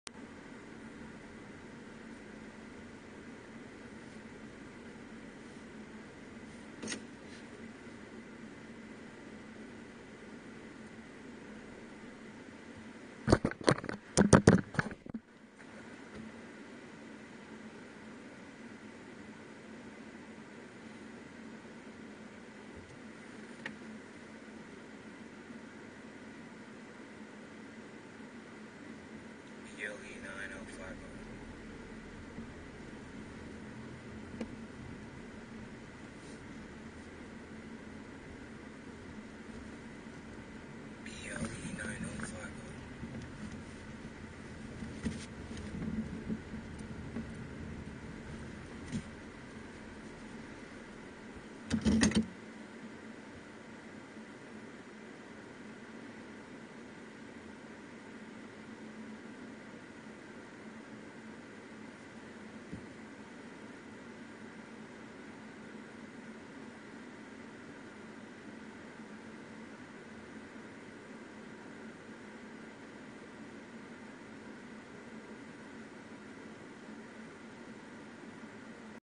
Almost road rage
Angry guy road rage caught on camera decided he didn't want to be filmed